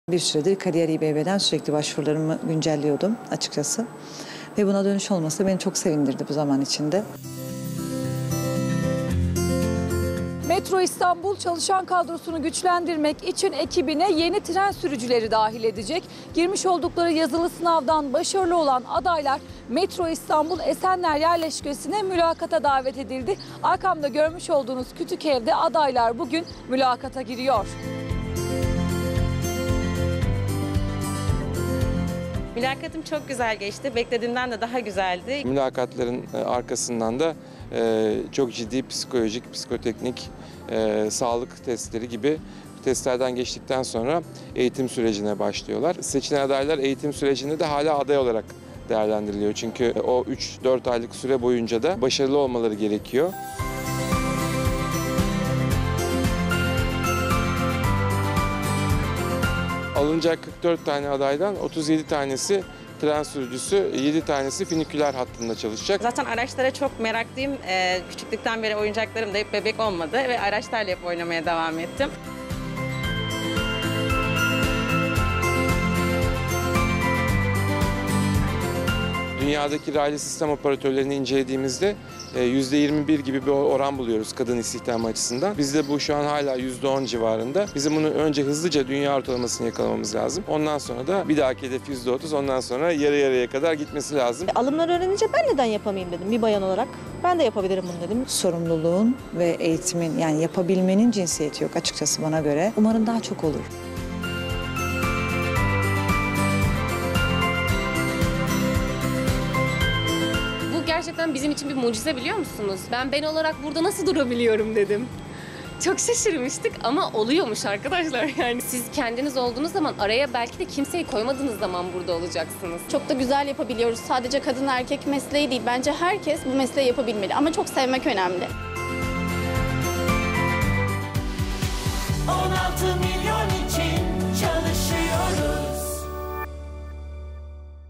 İBB’de kadın tren sürücülerine yenileri eklenecek
44 yeni kadın sürücü, Metro İstanbul bünyesinde M1, T1, M9 ve F4 hatlarında görev yapacak.

ayrıca İBBTV’yi diğer mecralarda da takip etmek için:
Makaleler, programlar ve canlı yayın: